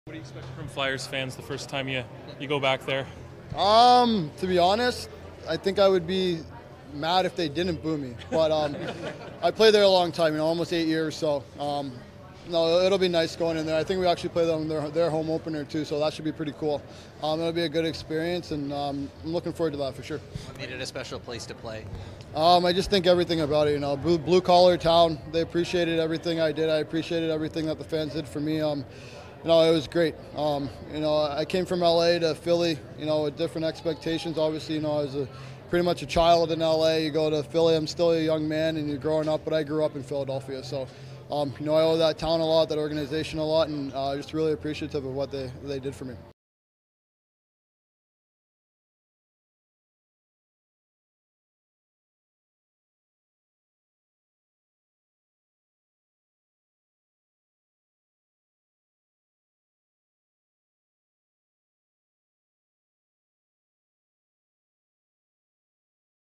Simmonds: I'd be disappointed if Flyers fans DON'T boo me
Having signed with the Devils this off-season, a redemption-minded Wayne Simmonds, jokes that he would be upset if the Flyers fans don't boo him when he returns to Philadelphia.